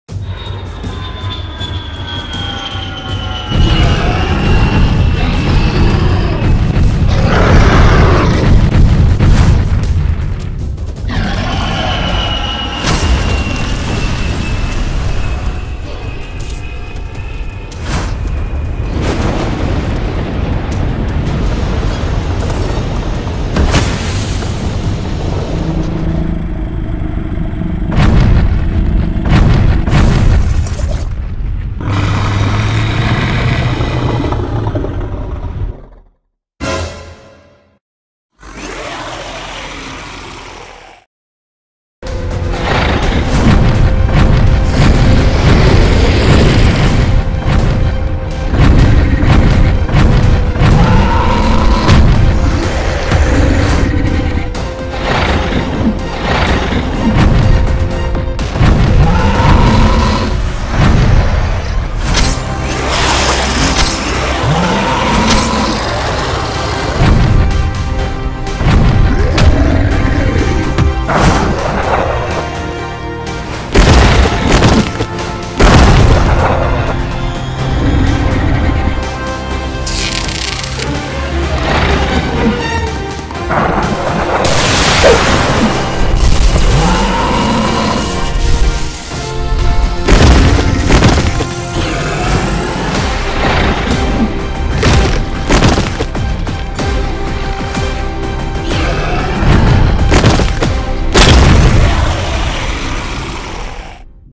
Shin Super Sonic vs Infected Sky 5 | Monster Animation
Shin Super Sonic from Shin Universe fight against Infected Sky 5 from Infected Sky Universe. Who will win in the fight between this two titan ? Let's find out with us in this video.

Featuring Sonic:
- Shin Super Sonic

Featuring Infected Sky: (by  @Complexanimations)
- Infected Sky 5